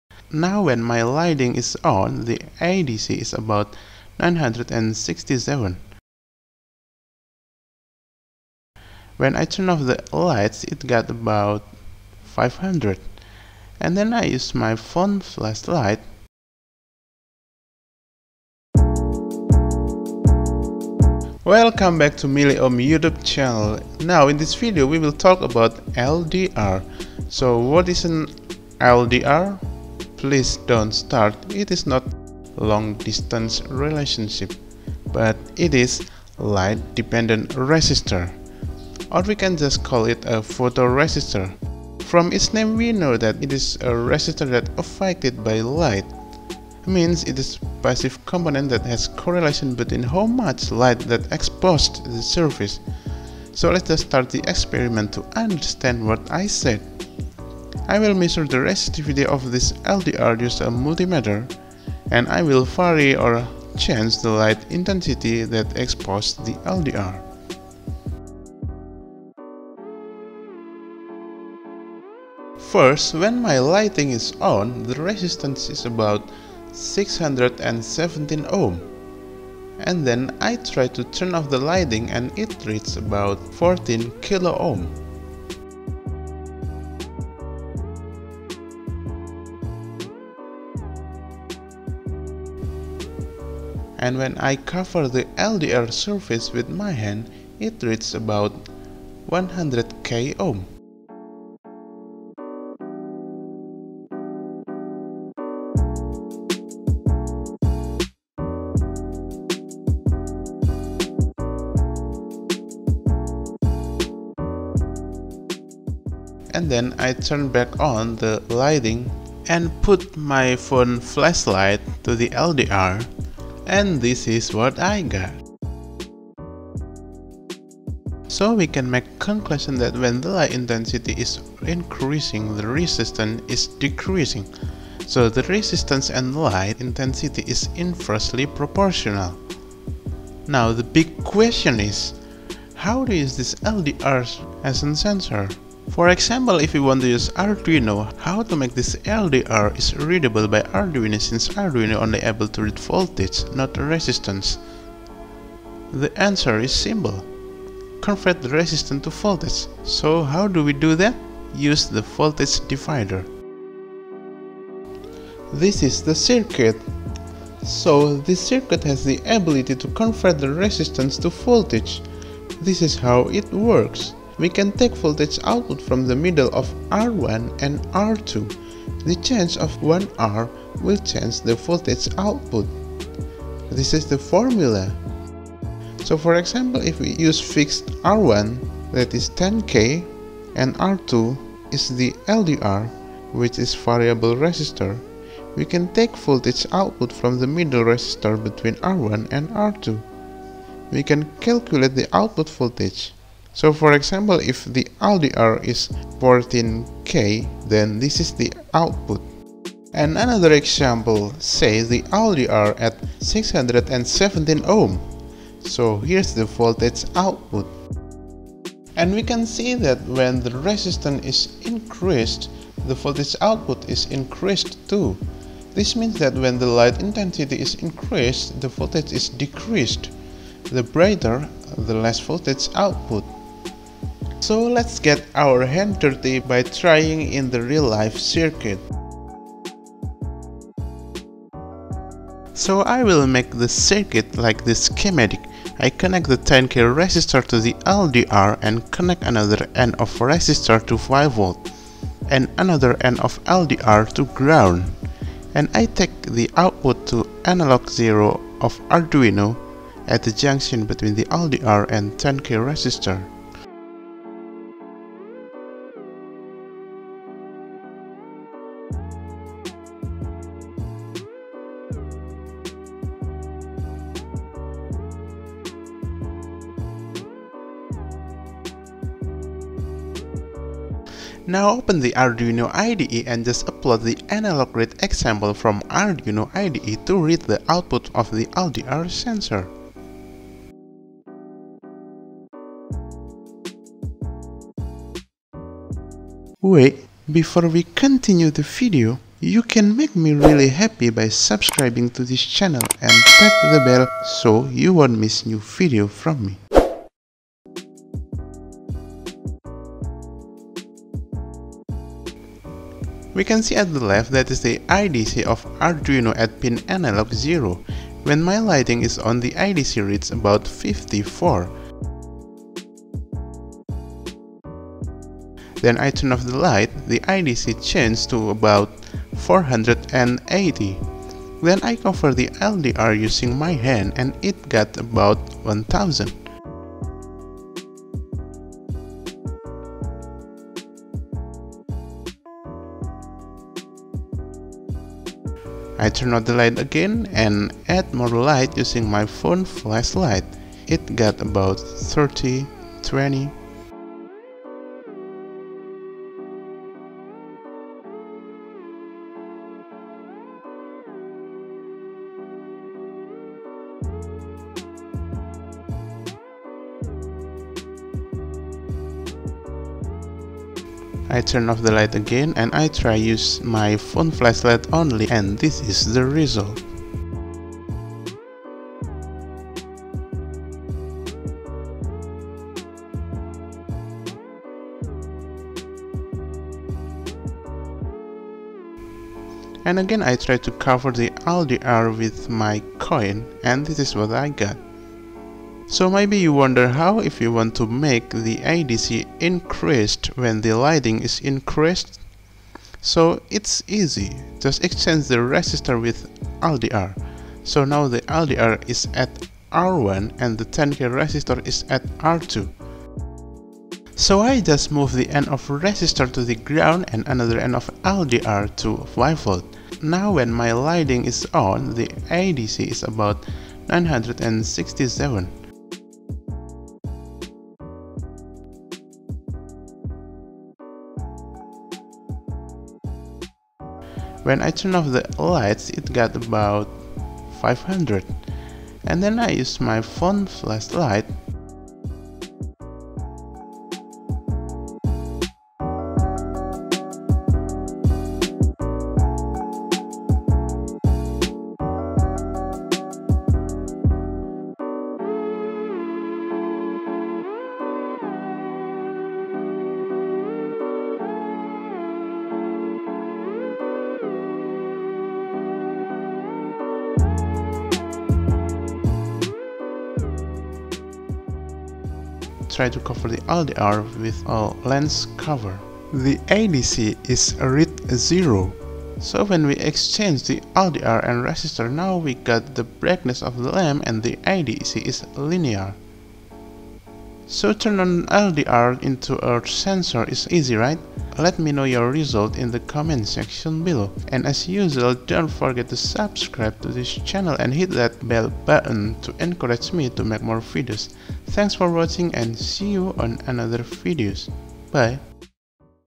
how to use LDR as a sensor ( Light intensity sensor with arduino tutorial )
LDR is a resistor that will change the resistance according to light that exposed to its surface. In this video we will learn how to use LDR as a light intensity sensor, then try to read the sensor using Arduino.